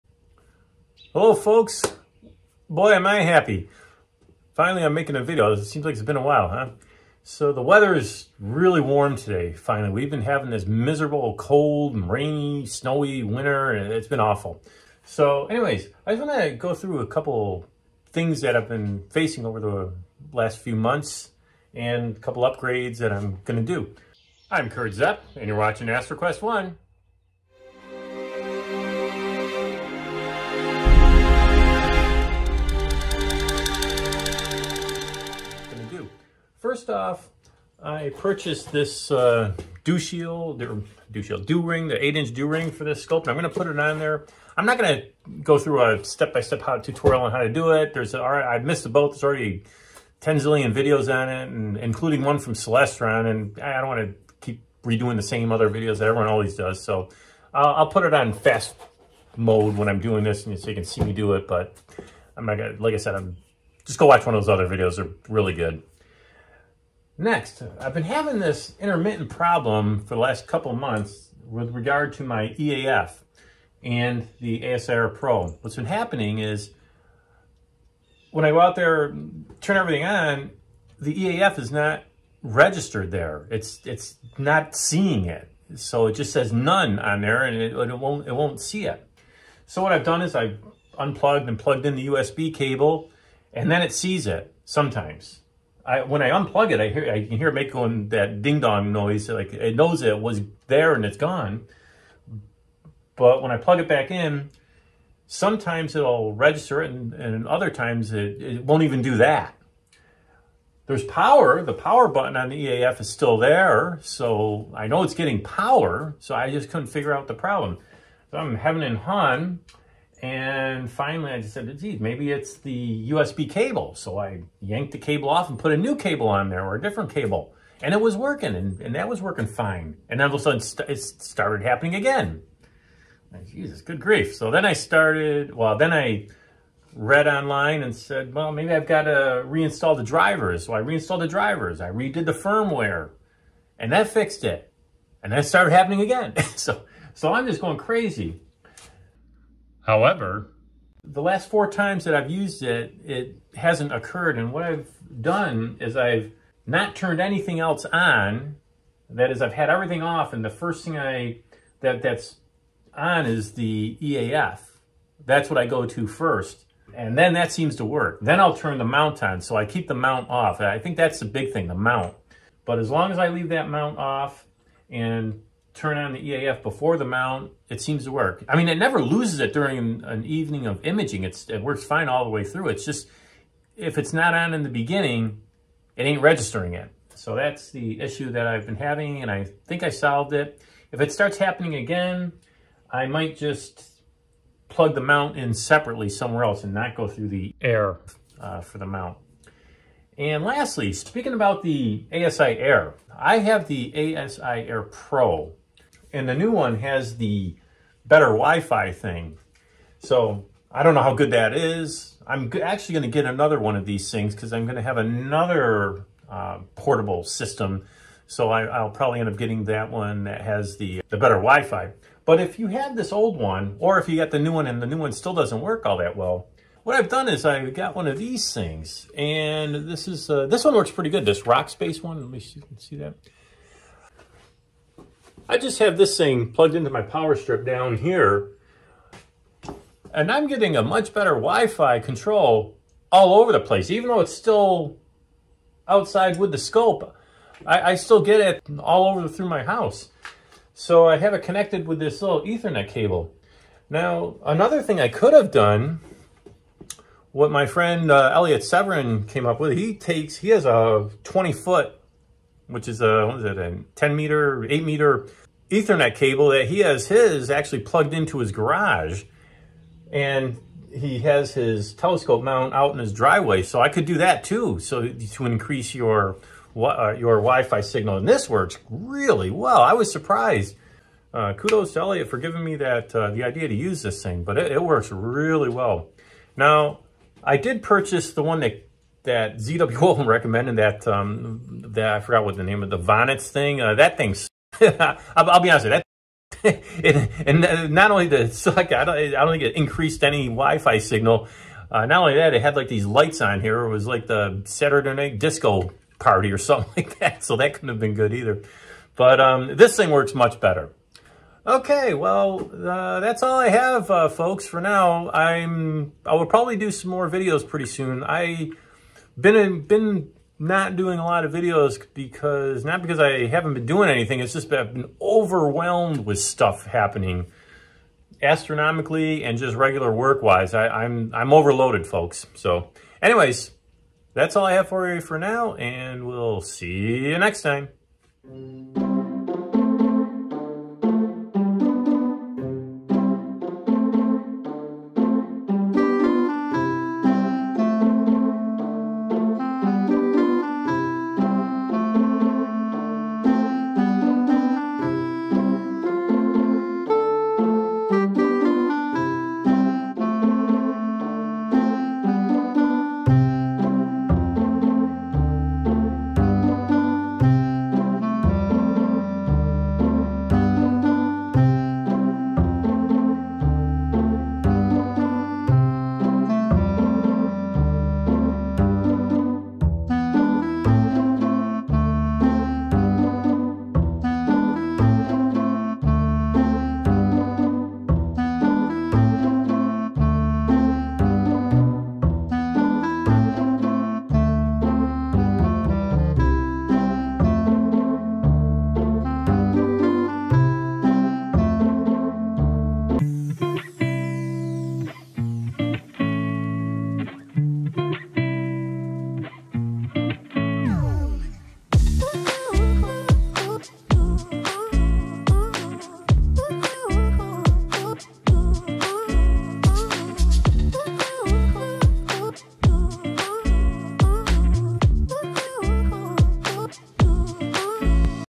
The Latest: Celestron Dew Heater Ring, EAF Connection Issue, ASIAIR Wifi Increase!
Finally the weather got a bit warmer so it's time to make a video.  I added the new Celestron Dew Heater Ring, fixed my intermittent EAF connection problem that I have been having, and substantially increased the Wifi range of my ASIAIR Pro.